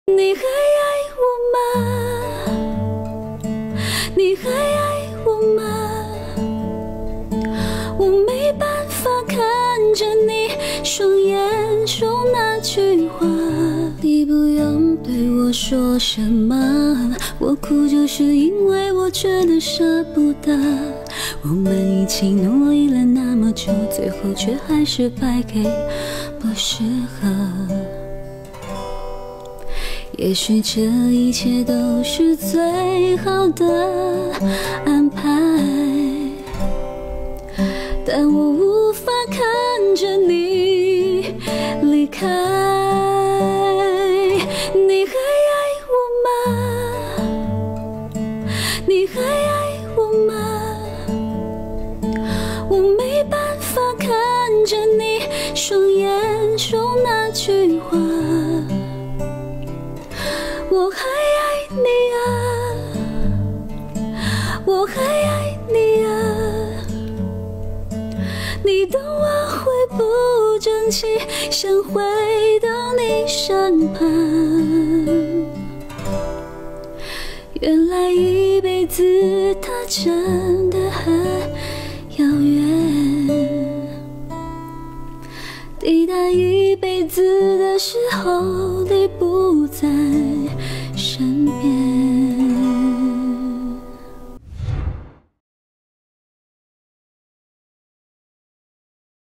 梁文音翻唱大陸節目爆紅神曲！ 深情詮釋網喊：心痛到睡不著｜TVBS娛樂頭條
影片來源：IG/liang_wen_yin